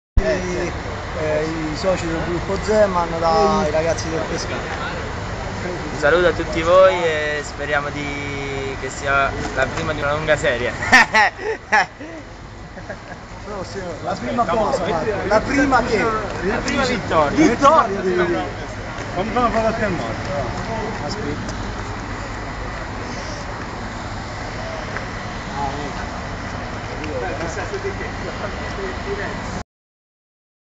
I ragazzi del Pescara...a Verona
Sansovini e Verratti del Pescara a Verona (partita contro l'Hellas vinta 2-1 dalla squadra di Zeman)